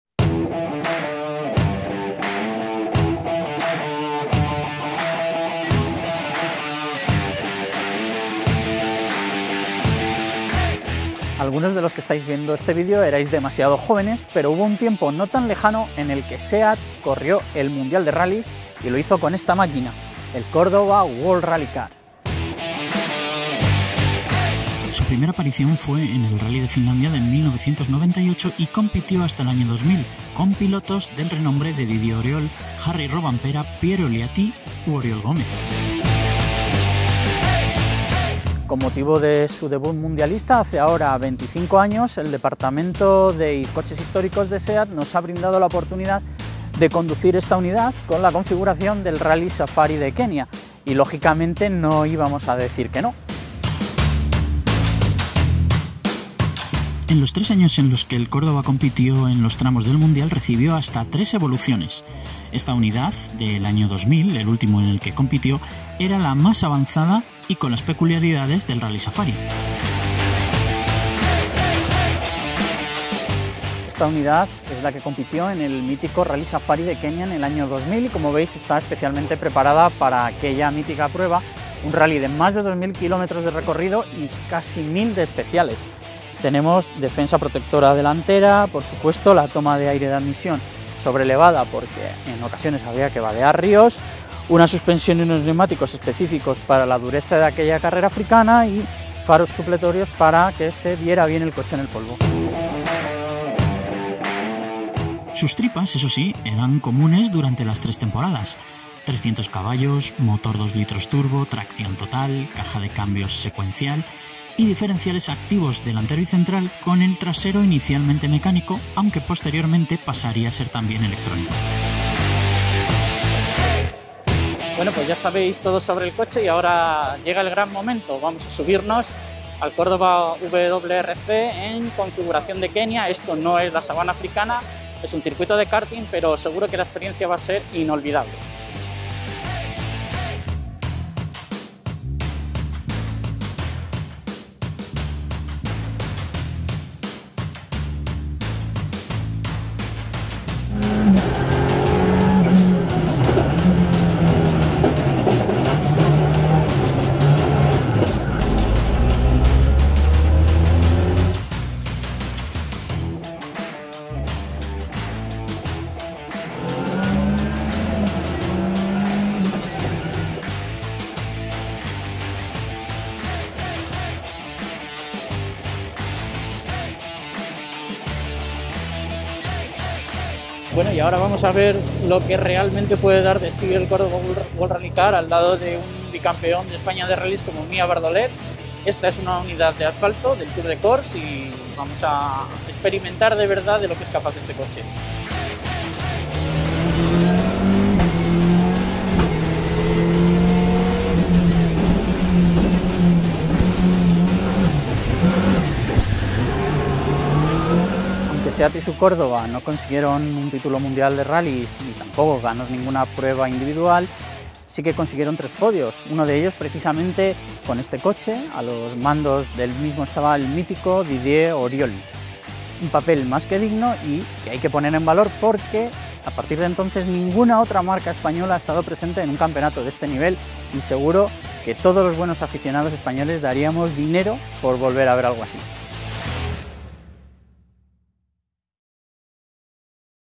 Seat Córdoba WRC: ¡lo probamos!
Se cumplen 25 años del debut de SEAT en la máxima categoría del Mundial de rallies. Fue con el Córdoba WRC, con el que compitió durante las temporada 1998, 1999 y 2000. Ahora, para celebrar las bodas de plata del coche español, hemos podido ponernos al volante y volver a una época dorada e irrepetible de los rallies.